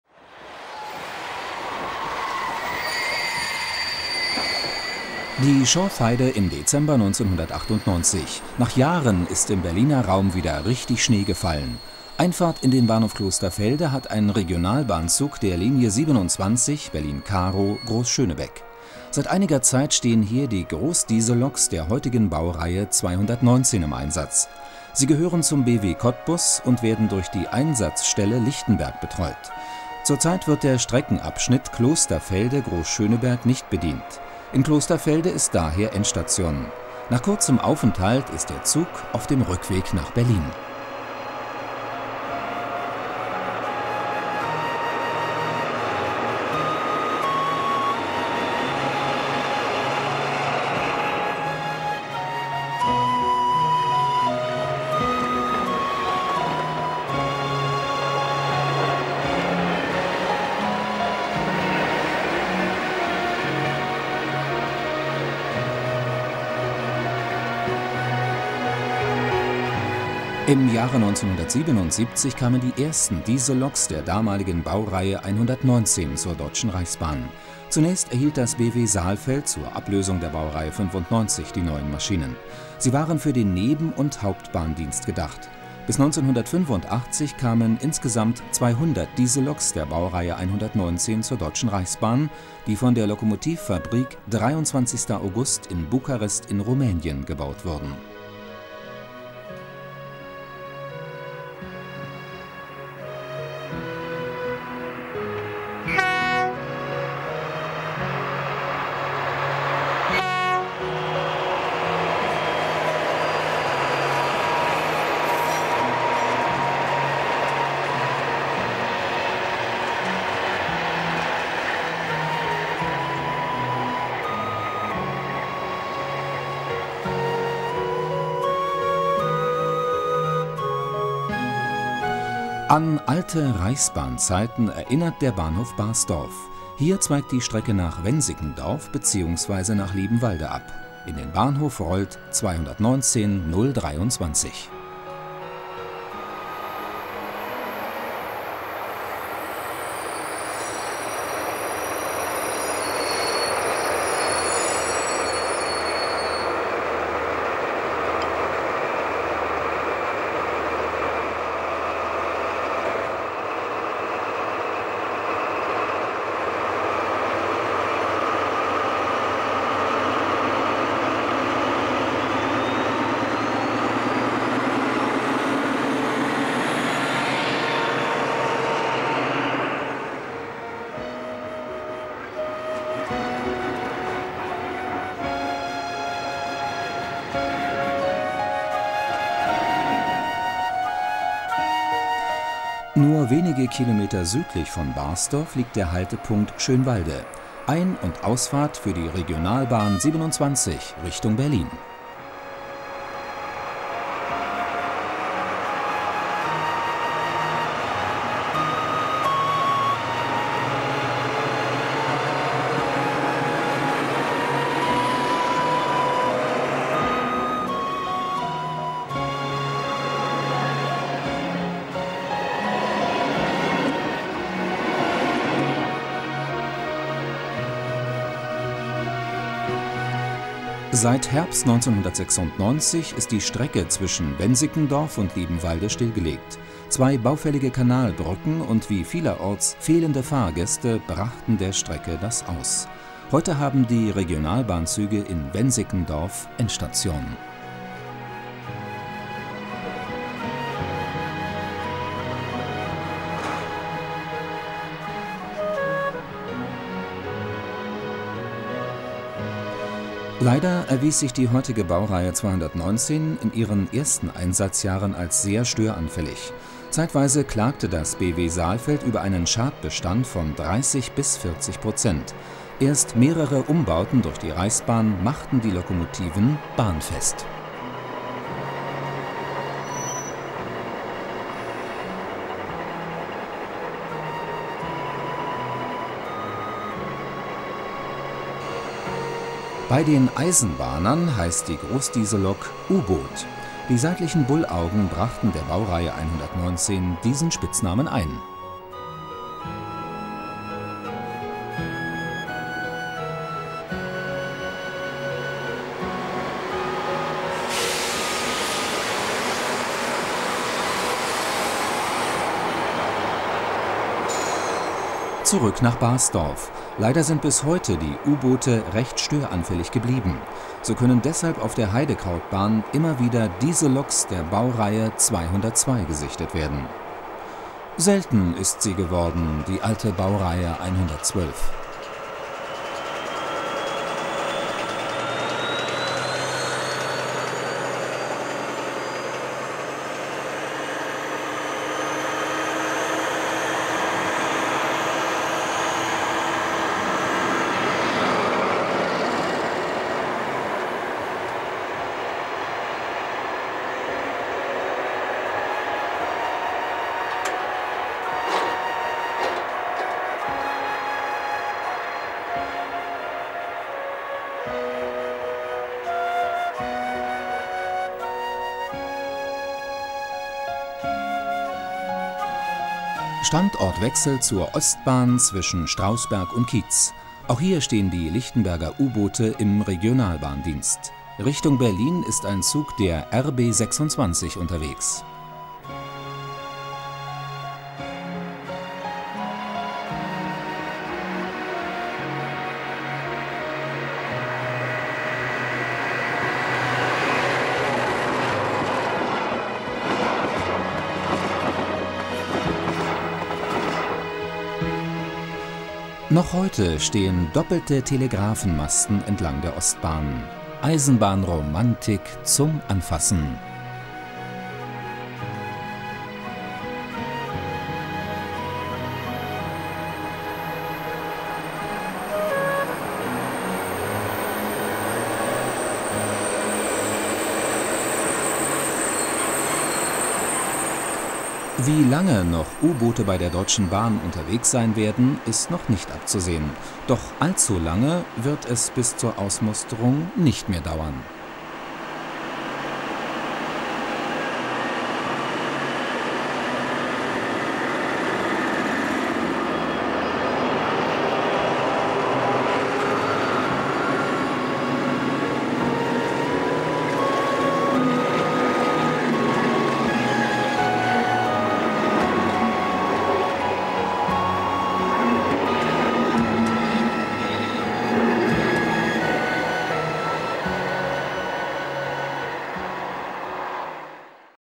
U-Boot oder Baureihe 119/219/229
Nach den Bestimmungen des RGW (Rat für gegenseitige Wirtschaftshilfe) durften in der DDR nur noch Dieselloks mit einer Leistung von bis zu 1.500 PS gebaut werden. So kam es, dass ab 1977 diese 2.700 PS starken Dieselloks aus Rumänien importiert wurden. Bei der Deutschen Reichsbahn wurden 200 Stück als Baureihe 119 eingeordnet. Die ersten Betriebsergebnisse waren deprimierend. Erst durch die sogenannte „Germanisierung“ – dem Tausch anfälliger Motoren und Bauteile gegen solche aus DDR-Produktion - konnten die Loks langsam ertüchtigt werden.